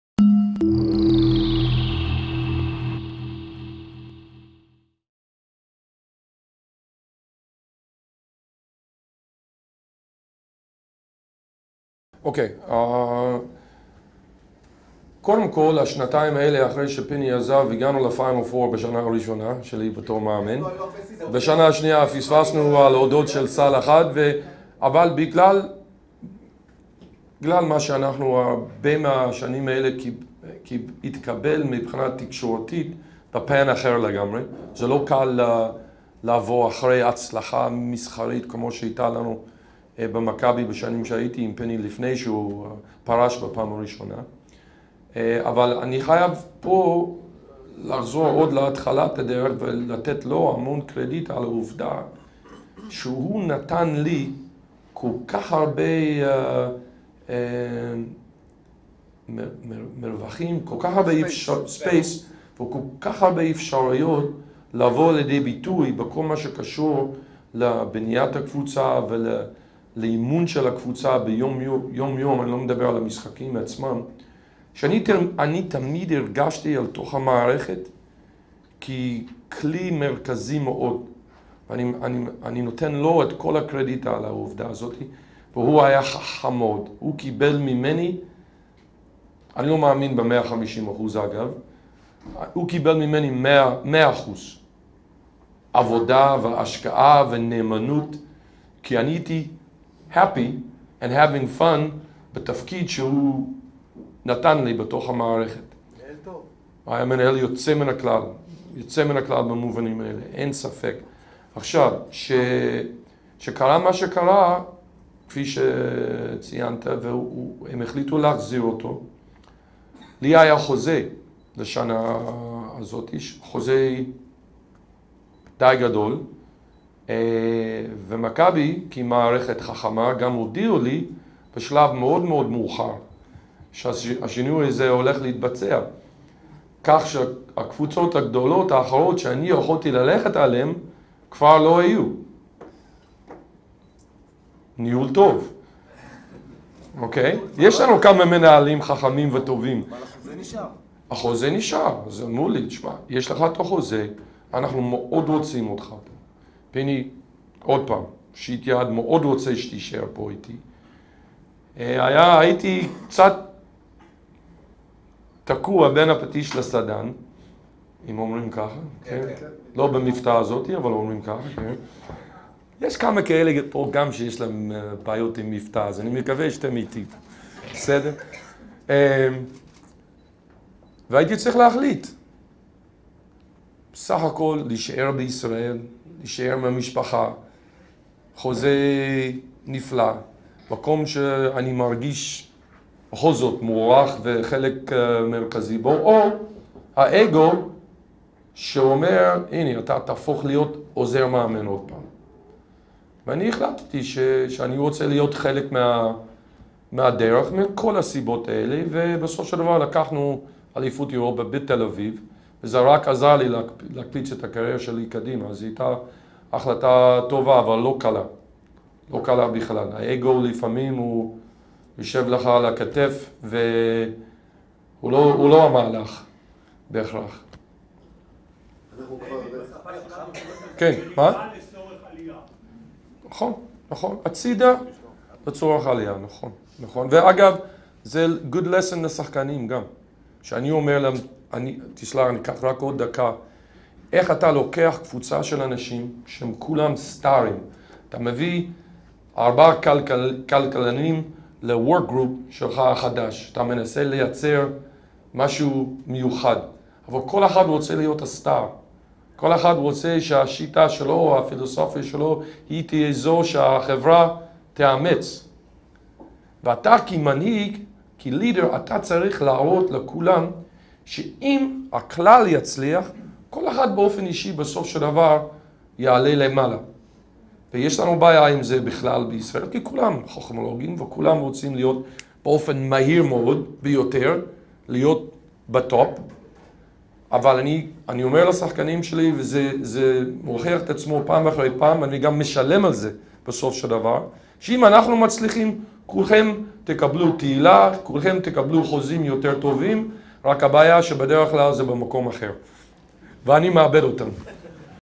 דיוויד בלאט על החזרה לעמדת עוזר המאמן
דיוויד בלאט עונה על השאלה "'למה הסכמת לנסיגה בקריירה- חזרה לעמדת עוזר מאמן ואיך מהלך זה תרם לך בהמשך הדרך?"

המדליסט האולימפי דיויד בלאט, מאמנה של נבחרת רוסיה ומכבי תל אביב בכדורסל הגיע לשיחה על סודות הניהול המנצח עם ראש ההתמחות החדשה באסטרטגיה ויזמות עסקית, ד"ר בועז שליט במסגרת היום הפתוח לתכנית ה-M.B.A בקריה האקדמית אונו